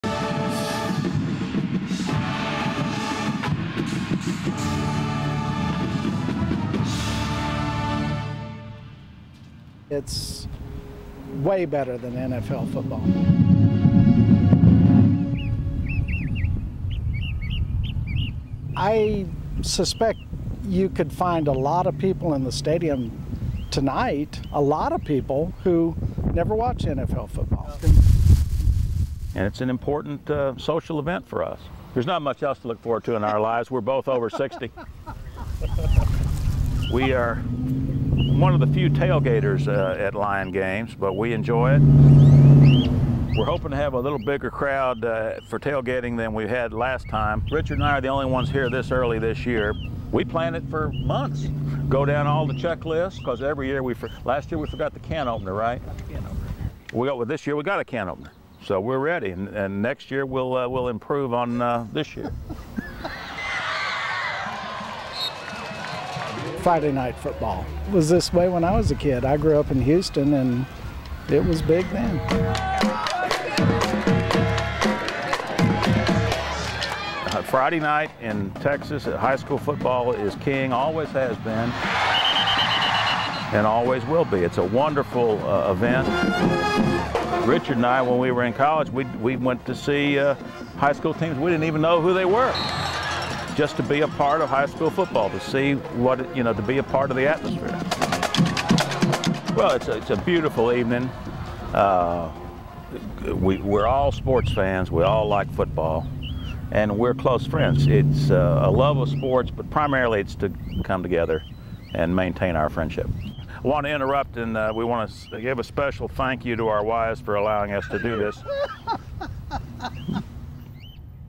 TAILGATING IN LEANDER
This is from October 2012, one of the very first things I produced myself in Texas. Most of these kids are probably 30-plus years old with three children by now. Not great, not my best work by a mile, and it's so slight it barely registers. But I like it, for nostalgic reasons.